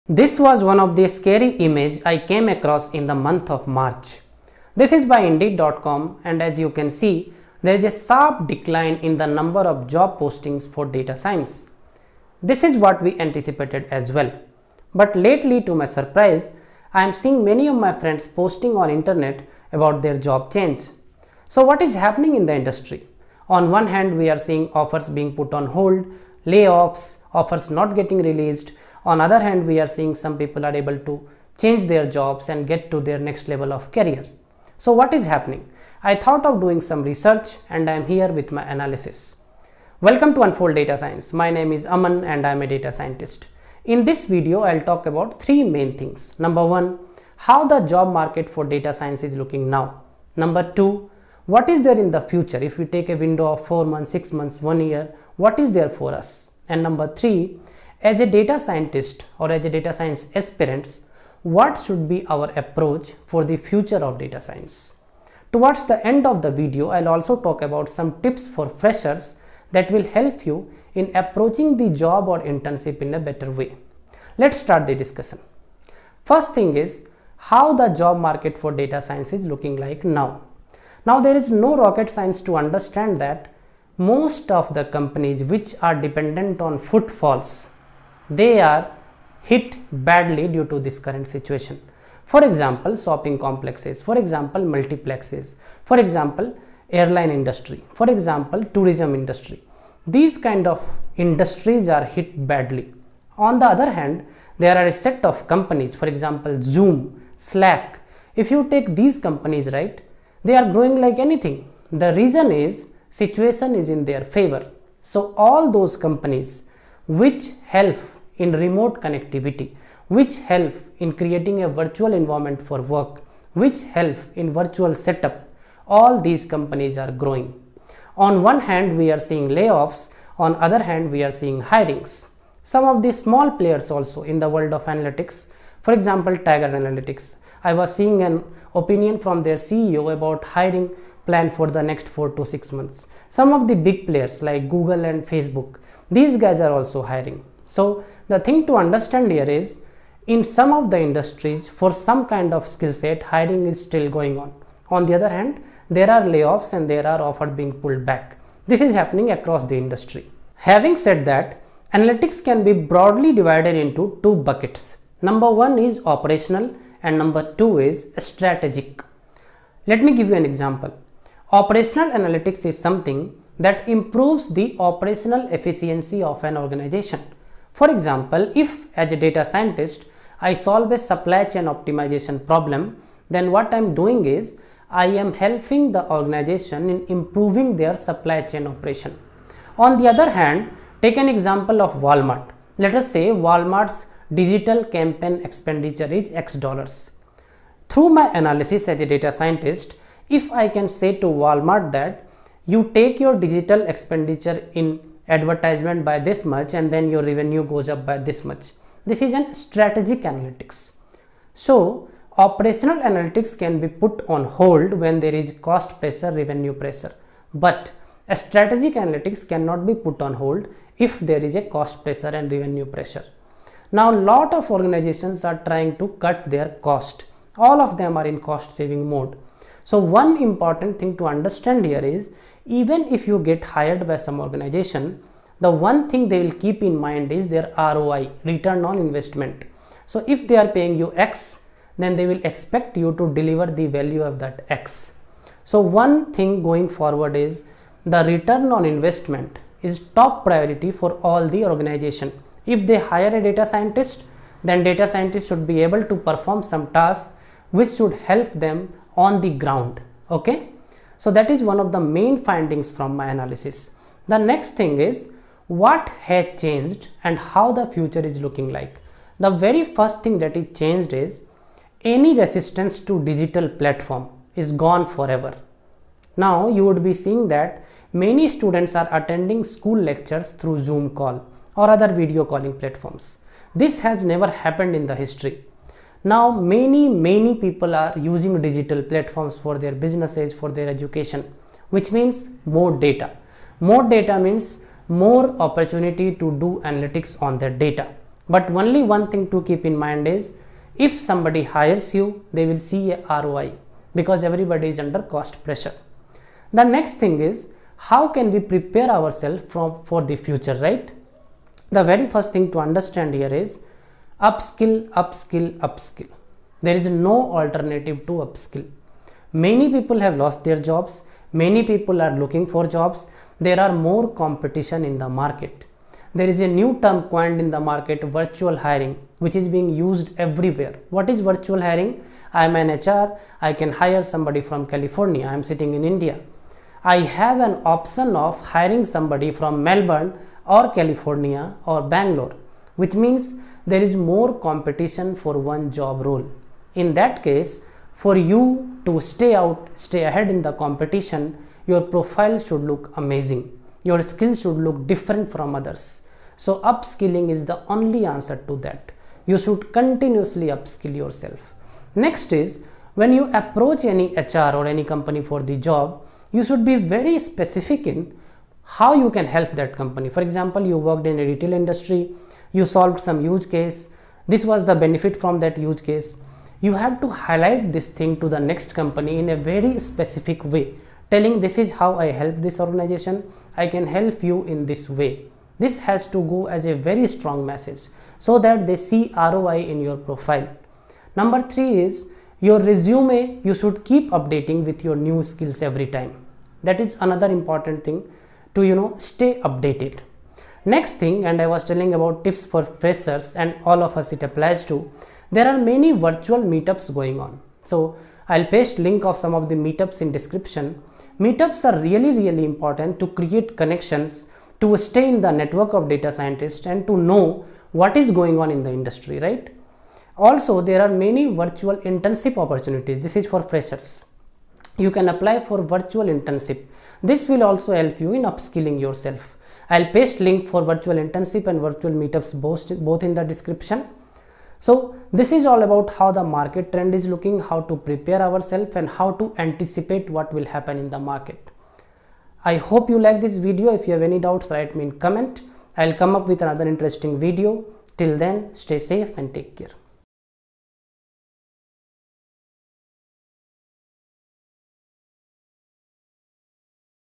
Data Science Job Market Now|How to get Data Science Job|Data Science Jobs
About this video:
In this video, I discuss about Data science job market in current market scenario. I discuss about how different industries are responding to current market scenario and how to apply jobs in data science in right way. I also discuss which industries are mostly impacted and which are less impacted.
Below questions are answered in this video:
1. How to apply for data science jobs?
2. Data Science market job market now?
3. Data Science job openings now?
4. Data science internship opportunities
5. Data science job trends